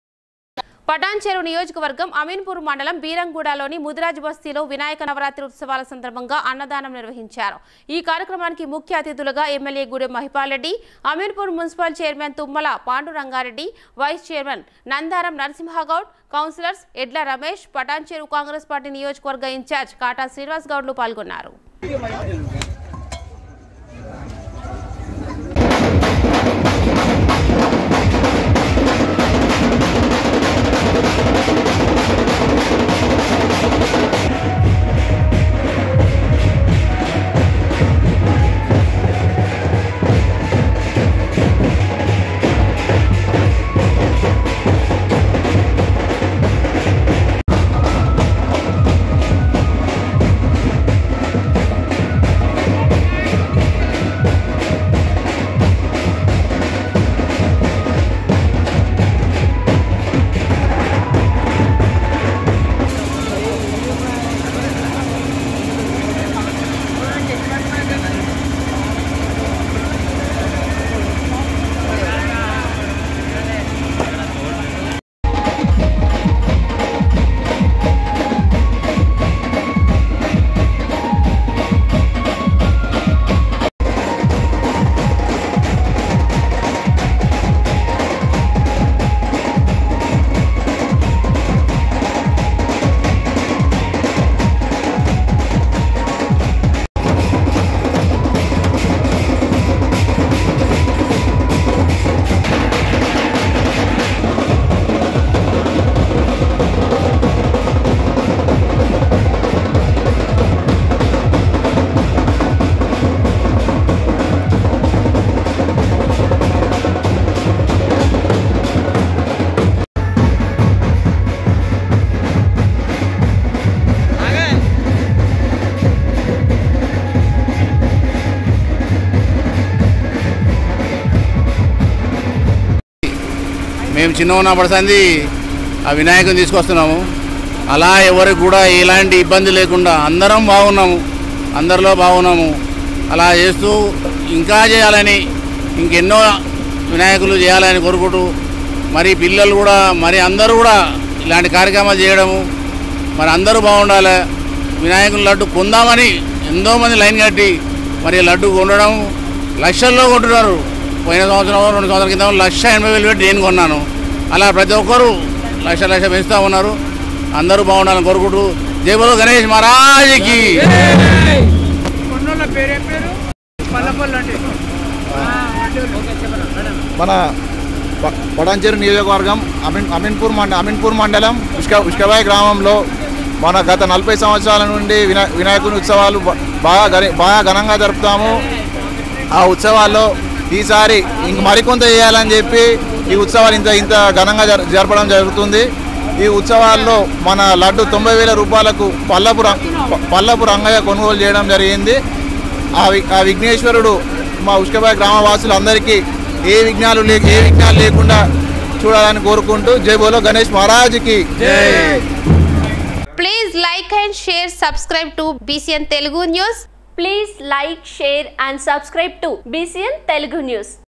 వినాయక నవరాత్రి ఉత్సవాల సందర్భంగా అన్నదానం నిర్వహించిన MLA గూడెం మహిపాల్ రెడ్డి
పటాన్చెరువు  నియోజకవర్గం అమీన్పూర్ మండలం బీరంగూడలోని ముదిరాజ్ బస్తీలో  వినాయక నవరాత్రి ఉత్సవాల సందర్భంగా అన్నదానం నిర్వహించారు .ఈ కార్యక్రమానికి ముఖ్య అతిథులుగా  ఎమ్మెల్యే గూడెం మహిపాల్ రెడ్డి, అమీన్పూర్ మున్సిపల్ చైర్మన్ తుమ్మల పాండురంగారెడ్డి , వైస్ చైర్మన్ నందారం నరసింహ గౌడ్, కౌన్సిలర్స్, ఎడ్ల రమేష్, పటాన్చెరువు కాంగ్రెస్ పార్టీ నియోజకవర్గ ఇన్చార్జి కాట శ్రీనివాస్ గౌడ్  లు పాల్గొన్నారు,
Annadanam was organized on the occasion of Vinayaka Navratri festival at Mudiraj Basti of Biranguda, Aminpur Mandal of Patancheruvu Constituency. MLA Goodem Mahipal Reddy, Aminpur Municipal Chairman Tummala Panduranga Reddy, Vice Chairman Nandaram Narasimha Goud, Councilors, Edla Ramesh, Pacheruvu Congress Constituency Constituency Incharge Katakani Goud participated as the chief guests